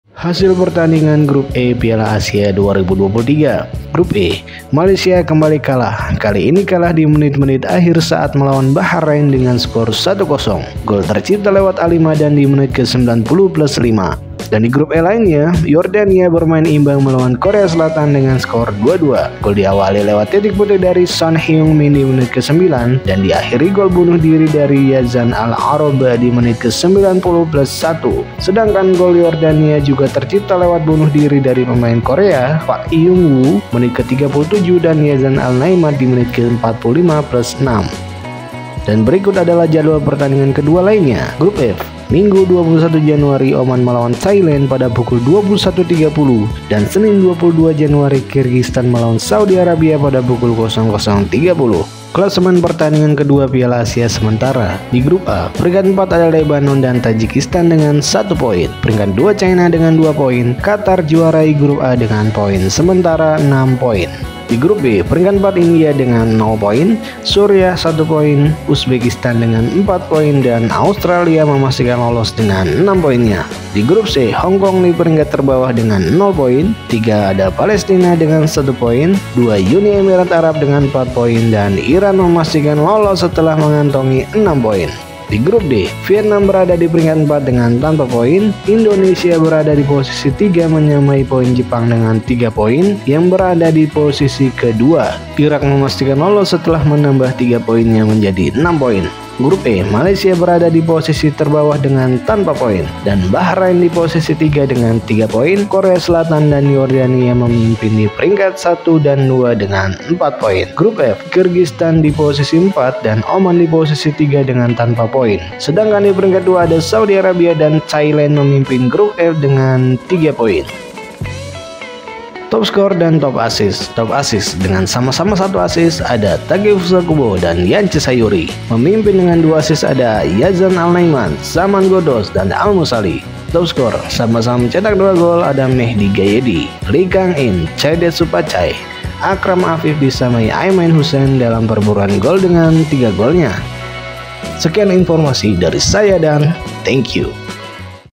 HASIL PIALA ASIA GRUP E | BAHRAIN 1-0 MALAYSIA | JADWAL KLASEMEN TOPSKOR ASIAN CUP 2023
Hasil Pertandingan Piala Asia AFC Asian Cup 2023 :
Pertandingan ke-2 :
Bahrain 1-0 Malaysia
Jordan 2-2 Korea Republic
Vietnam 0-1 Indonesia
Iraq 2-1 Japan
Hongkong 0-1 Iran
India 0-3 Uzbekistan
Palestine 1-1 Uni Emirat Arab
Syria 0-1 Australia
Tajikistan 0-1 Qatar
Lebanon 0-0 China

Pertandingan ke-1 :

Qatar 3-0 Lebanon
China 0-0 Tajikistan
Australia 2-0 India
Uzbekistan 0-0 Syria
Uni Emirate Arab 3-1 Hongkong
Iran 4-1 Palestine
Japan 4-2 Vietnam
Indonesia 1-3 Iraq
Korea Republic 3-1 Bahrain
Malaysia 3-0 Jordan
Thailand 2-0 Kyrgyztan
Saudi Arabia 2-1 Oman

Di Piala Asia atau AFC Asian Cup 2023 Qatar ini menghadirkan 24 negara terbaik asia yang memperebutkan 2 peringkat terbaik di masing-masing grup dan memperebutkan 4 tim 3 terbaik untuk melengkapi 16 Tim yang akan melangkah ke fase knockout. Dan berikut adalah negara di tiap-tiap Grup :
Grup A Ada Qatar, China, Tajikistan, Lebanon
Grup B Ada Australia, Uzbekistan, Syria, India
Grup C Ada Iran, Uni Mirat Arab, Hongkong, Dan Palestina
Grup D Ada Jepang, Indonesia, Iraq Dan Vietnam
Grup E Ada Korea Selatan, Malaysia, Yordania Dan Bahrain
Grup F Ada Saudi Arabia/,Thailand, Kirgistan Dan Oman